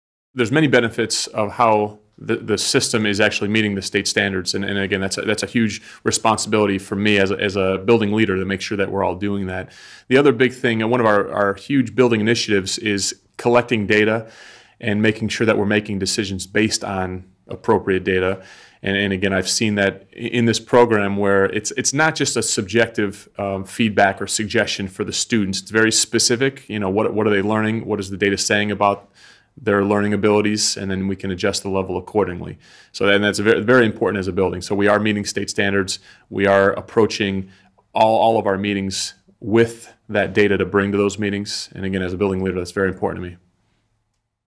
Adam Dudziak on Collecting Data | n2y
Adam Dudziak from Memorial Middle School discusses how Unique Learning System has helped them with data collection.